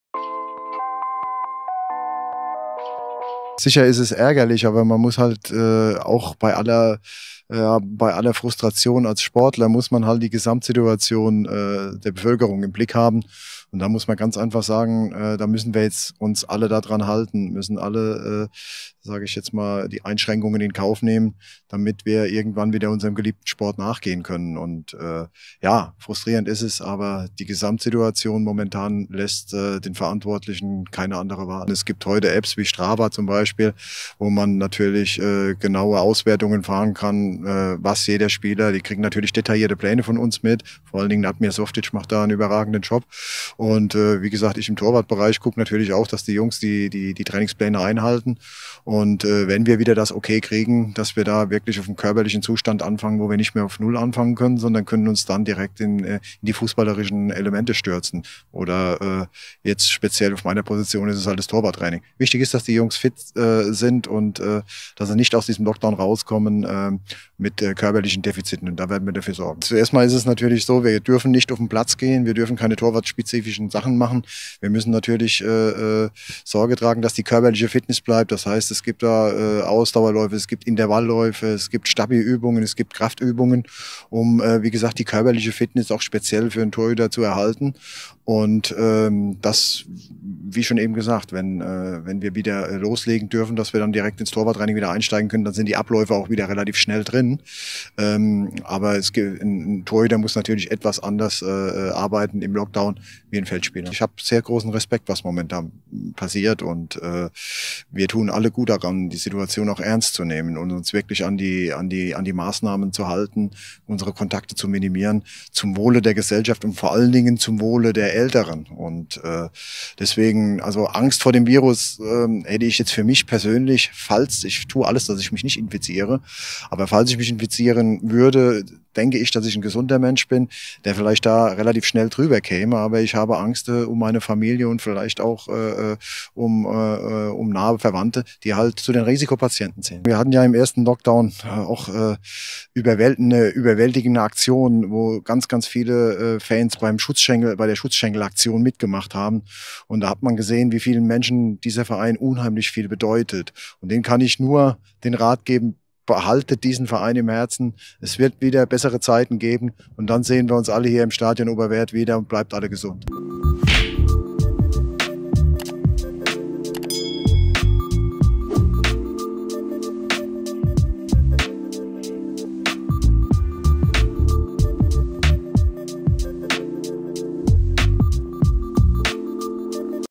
ACCESS ALL AREAS X TuS Koblenz : Peter Auer über Corona (Bonus)
Auch für den TuS Koblenz - Torwarttrainer Peter Auer stellt Corona eine enorme Belastung dar. Wie soll er seine Torwarte trainieren? Wie sicher ist seine Familie? Antworten auf diese Fragen liefert Peter in diesem Interview.

you ain't seen nothin yet.
mutant movement til we rot.
ad astra per aspera, kimosabe.